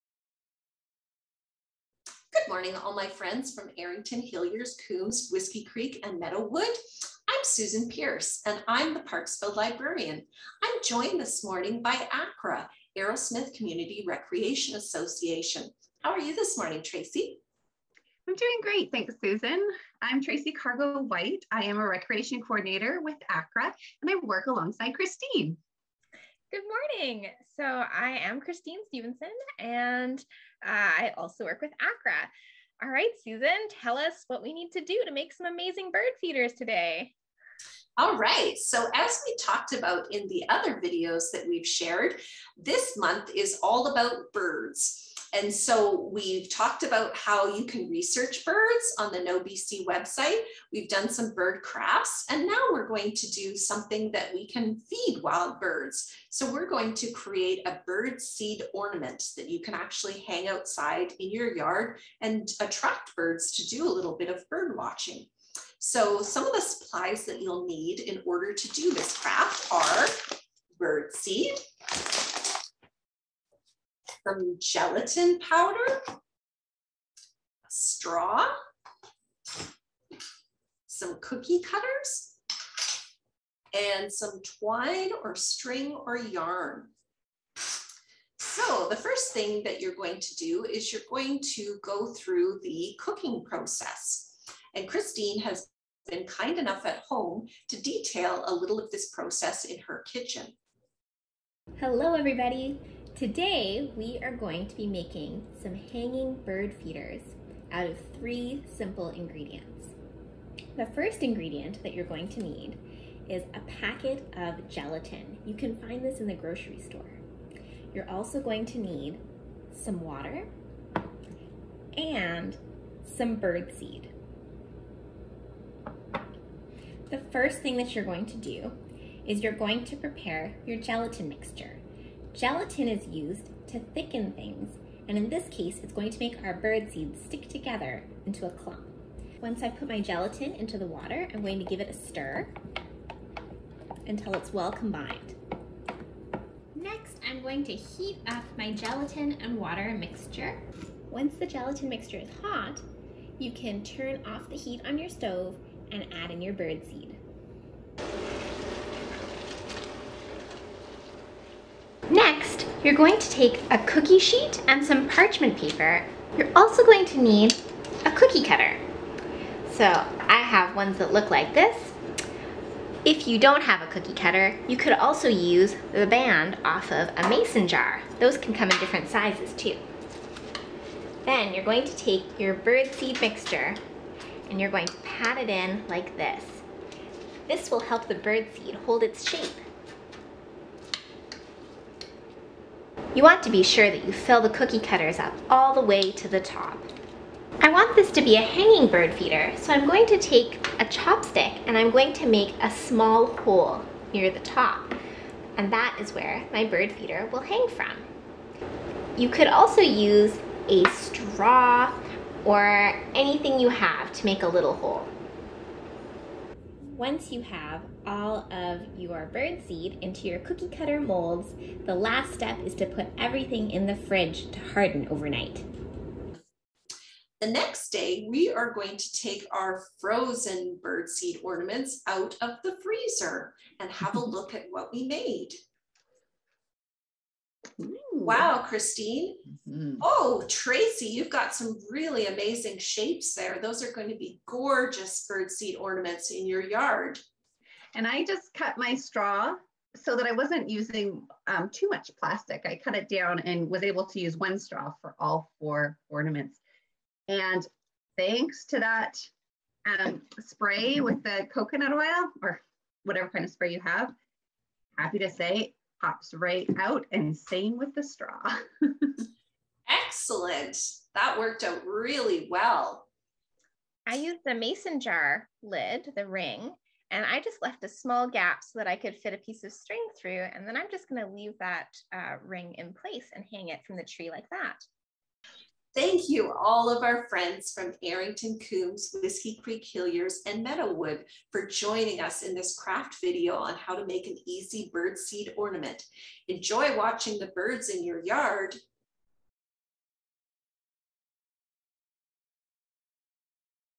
Hanging Bird Feeders
Attract wild birds to your yard with these homemade bird feeders. This activity requires a stove and three simple ingredients.

Intended for ages 3+.
Adult assistance required.